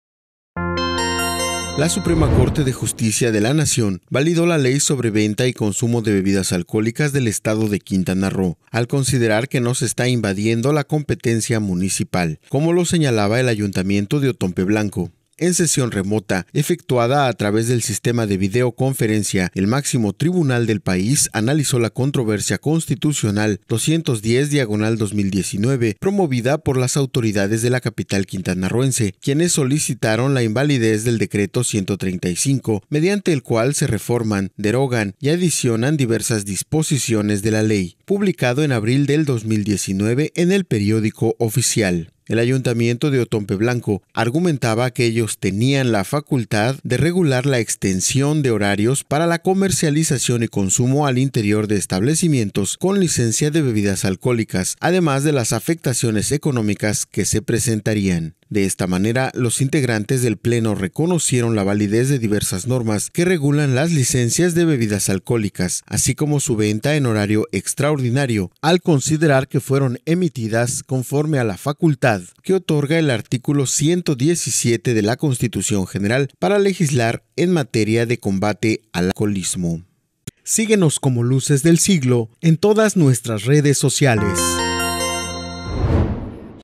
Da Suprema Corte luz verde a ‘Ley de alcoholes’
La Suprema Corte de Justicia de la Nación (SCJN) validó la Ley sobre Venta y Consumo de Bebidas Alcohólicas del estado de Quintana Roo, al considerar que no se está invadiendo la competencia municipal, como lo señalaba el Ayuntamiento de Othón P. Blanco.